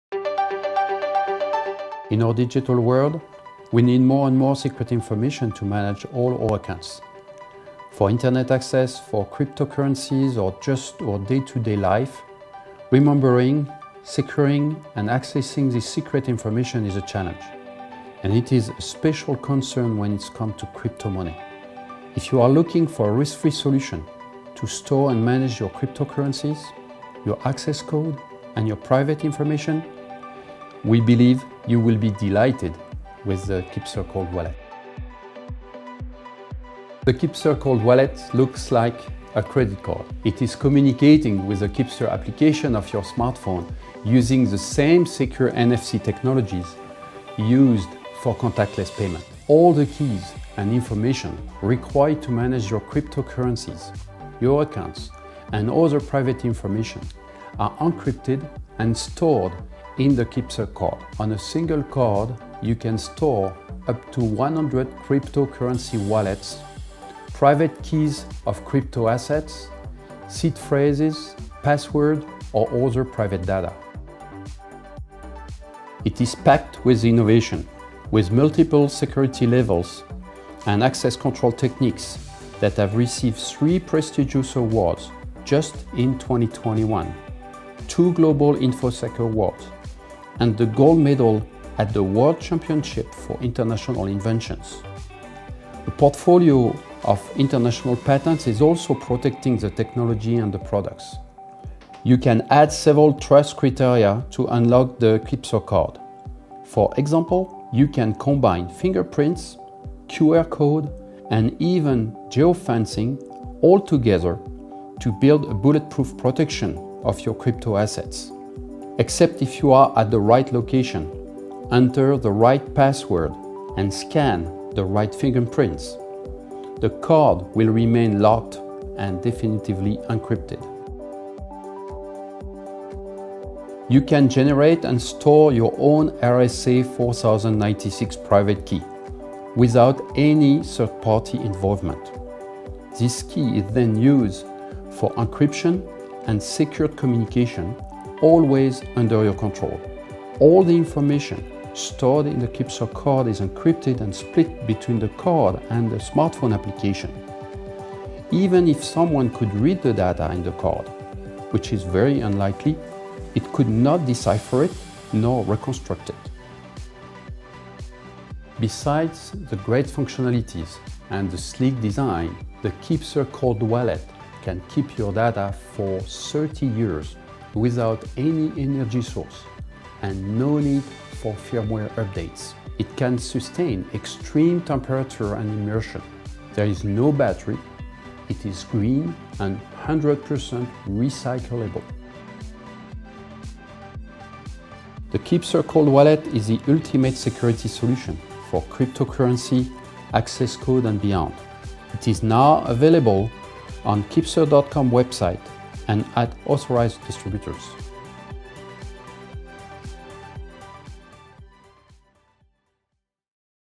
Product Presentation - Keepser Cold Wallet
In our modern digital world, a growing number of secret information is required to manage all our accounts, our cryptocurrencies, or our private life in general. Remembering and securing all this secret information has become a challenge, and a special concern when it comes to high-value crypto-assets.

If you are looking for a risk-free solution to store and manage your crypto-currencies, your access codes, and your private information, you will be delighted by the Keepser Cold Wallet.

The Keepser Cold Wallet looks like a credit card. It is communicating with the Keepser application on your smartphone, using the same secured NFC technology used for contactless payments. All the keys and information required to manage your cryptocurrencies, your accounts, and other private information are encrypted and stored in the Keepser card. In a single card, you can store up to 100 cryptocurrency wallets, private keys of crypto-assets, "seed phrases", passwords or other private data.